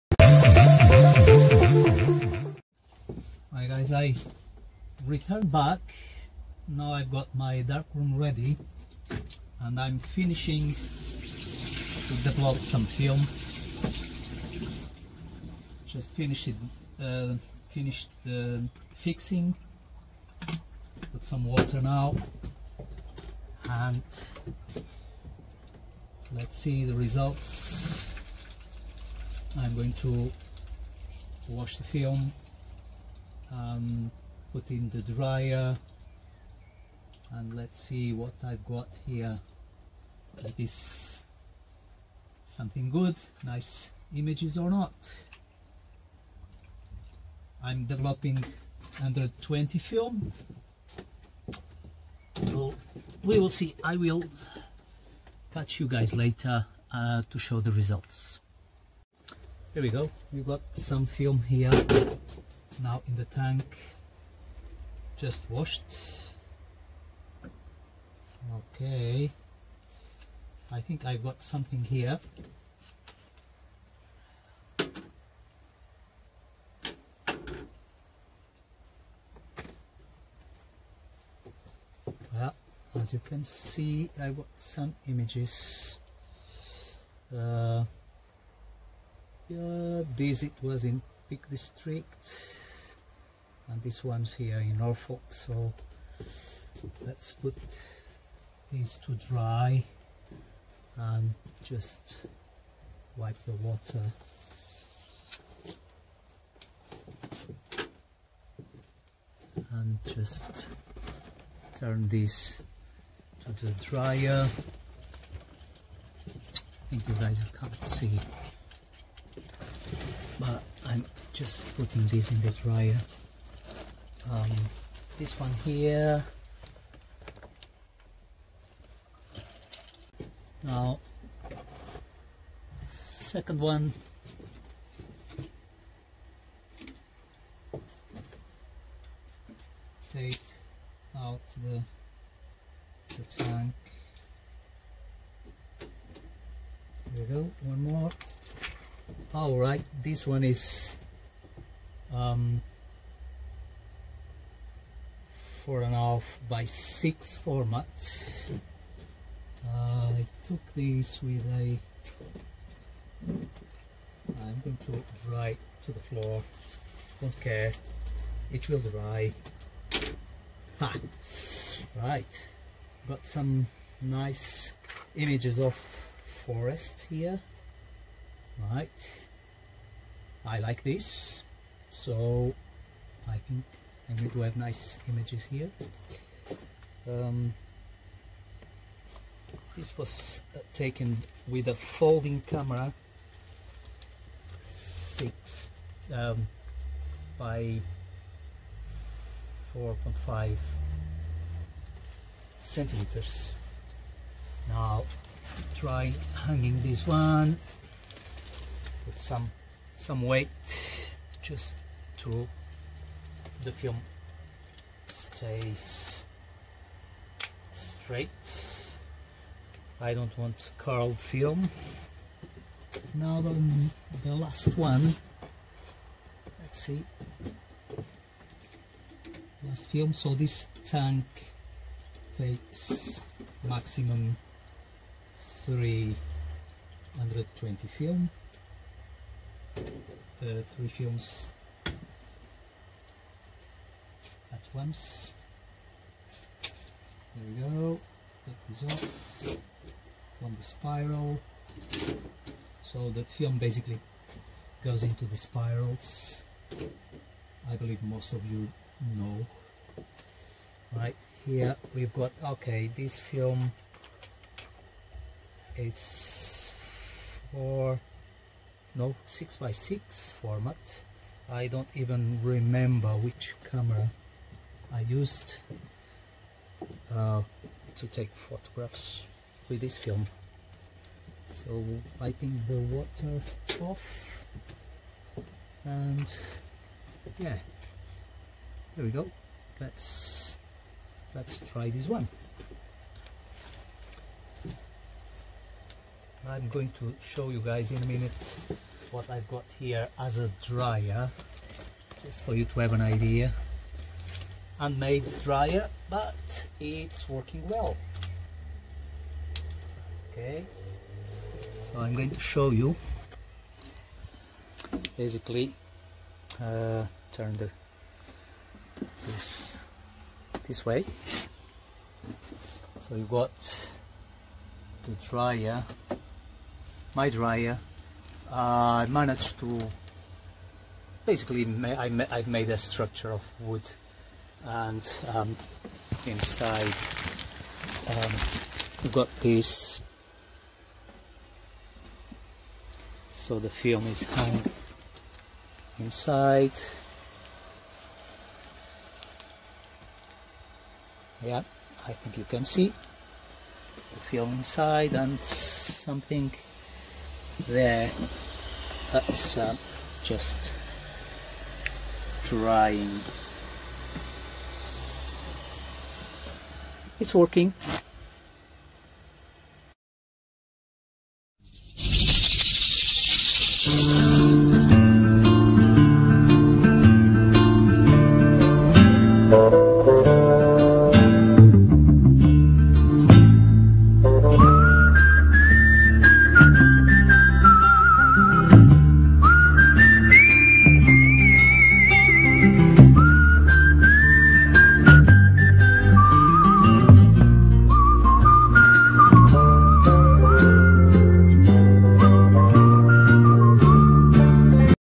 Developing and drying 120 film
My first developing in my brand new darkroom. 120 black and white film developing and drying.